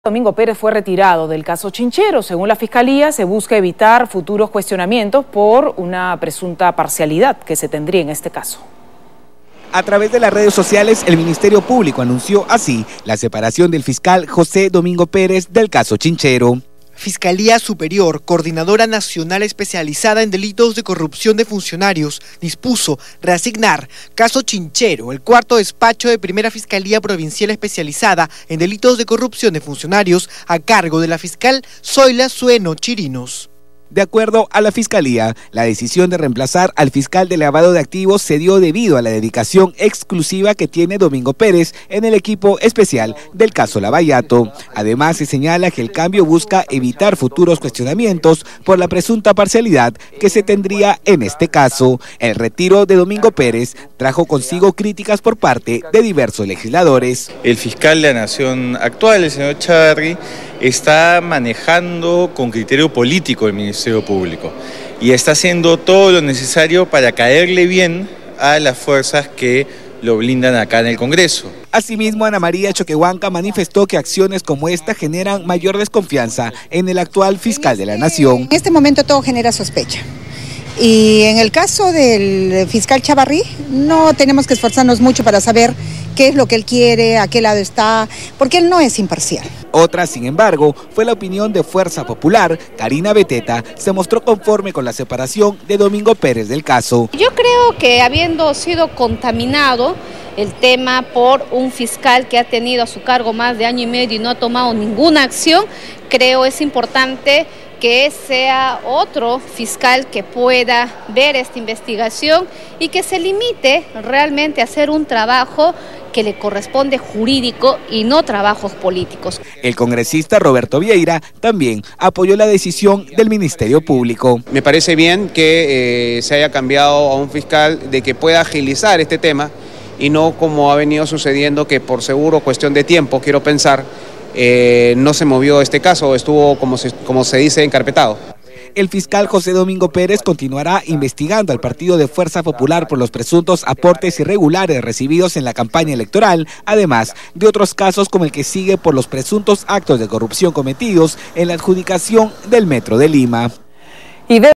Congresistas se pronuncian tras separación de fiscal Domingo Pérez de Caso Chinchero
Según la Fiscalía de la Nación, esta decisión se dio debido a la dedicación exclusiva que tiene Domingo Pérez en el equipo especial del caso Lava Jato, además se señala que el cambio busca evitar futuros cuestionamientos por la presunta parcialidad que se tendría en este caso.

Esta medida trajo consigo críticas por parte de diversos legisladores en el Congreso de la República. Ana María Choquehuanca manifestó que acciones como esta generan mayor desconfianza en el actual fiscal de la Nación, Pedro Chávarry.

Por su parte el congresista Roberto Vieira sí apoyó la decisión del Ministerio Público a fin de agilizar el proceso.

Cabe resaltar que el fiscal José Domingo Pérez continuará investigando al partido Fuerza Popular por los presuntos aportes irregulares recibidos en la campaña electoral, además de otros casos como el que sigue por los presuntos actos de corrupción cometidos en la adjudicación del Metro de Lima.